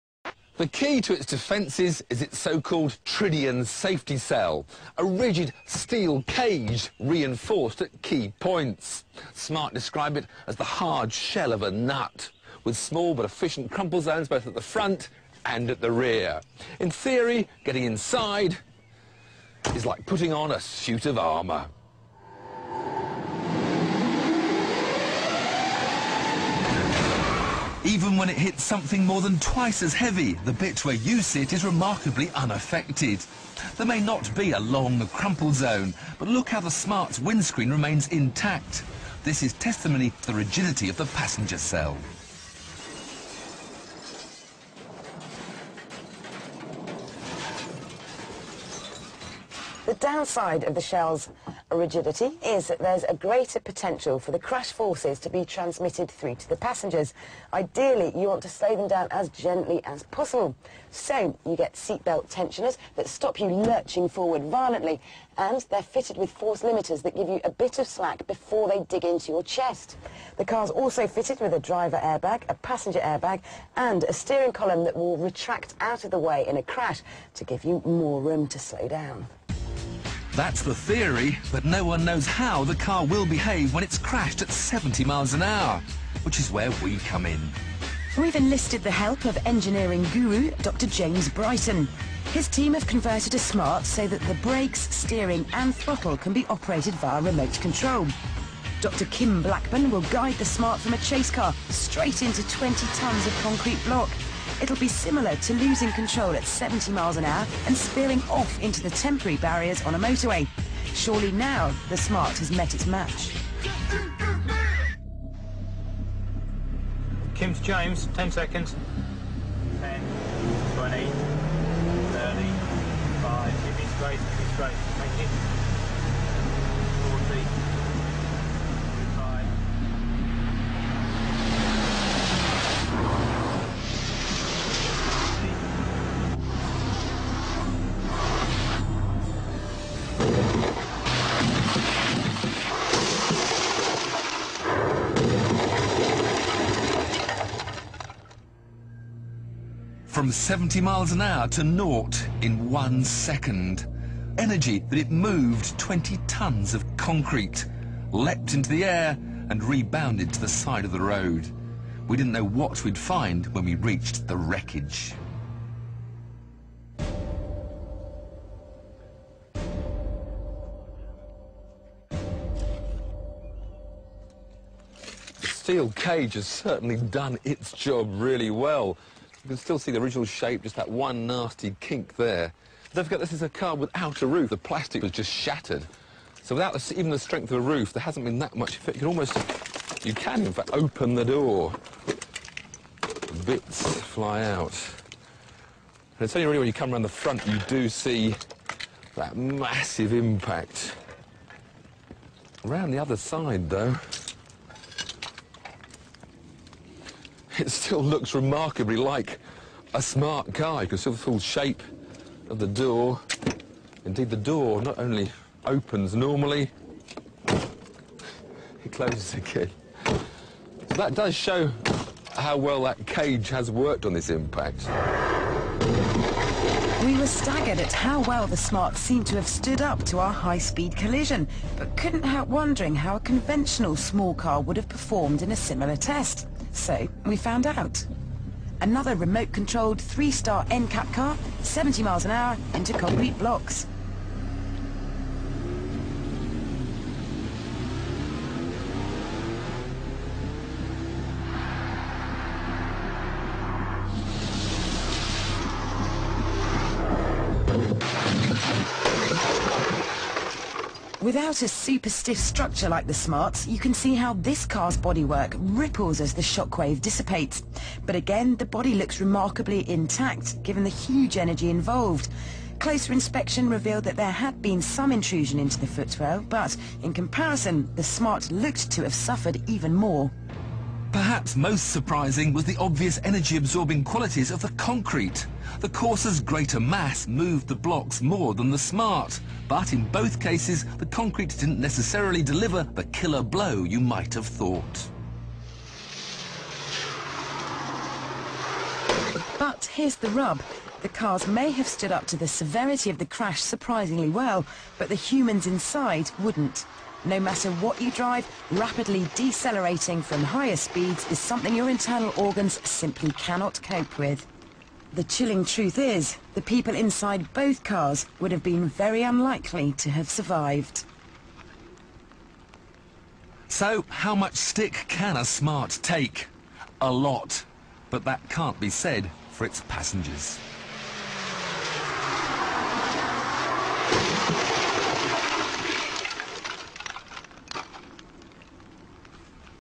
The Entire Smart Fortwo Crash Test
I'm re-posting this purely as a response to the extremely dubious edited version of this video that sugarcoats, nay, completely whitewashes the results of this test in a positive light.

The Smart Fortwo is not a safe vehicle. It has next-to-no crumple zones. It does not matter what you hit, be it a Hummer, a tractor-trailer, a tree or another Smart Fortwo. Crumple zones absorb energy from a collision and allow your fragile human body to slow down (relatively) gradually.

Notice, the Corsa in the video has a front crumple zone that absorbs energy at impact. It has less chassis intrusion than the Smart and would have subjected its occupants to less crash forces.

As Tiff points out in the video, the concrete barriers are actually more forgiving than they appear.. as they absorb some of the crash forces by moving. The vast majority of things you might hit with a Fortwo (larger vehicles and immovable objects) would show even worse results.

The Smart Fortwo was designed to make perpendicular parking in parallel parking spots possible in dense urban areas where traffic moves slowly. The Fortwo is not unsafe in the USA because there are large cars and SUVs on the road. It is unsafe because it was never meant to be used regularly at high speeds like American cars are.

Friends don't let friends delude themselves and others about the Fortwo's safety.

Transcript of the conclusion by Tiff Needel and Vicki Butler Henderson:

Tiff: Perhaps most surprising was the obvious energy absorbing properties of the concrete. The Corsa's greater mass moved the blocks more than the Smart; but in both cases the concrete didn't necessarily deliver the killer blow you might have thought.

Vicki: But, here's the rub. The cars may have stood up to the severity of the crash surprisingly well, but the humans inside wouldn't. No matter what you drive, rapidly decelerating from higher speeds is something your internal organs simply cannot cope with. The chilling truth is: the people inside both cars would have been very unlikely to have survived.

Tiff: So, how much stick can a Smart take? A lot; but that can't be said for its passengers.